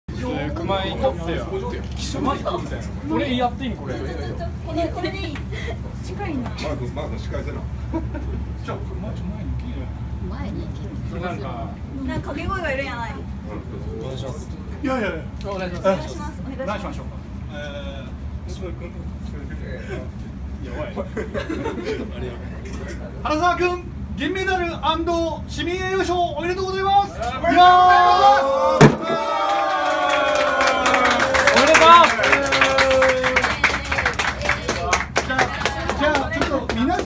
原沢久喜選手凱旋祝勝会鏡割り
リオデジャネイロ五輪銀メダル&市民栄誉賞おめでとうございます。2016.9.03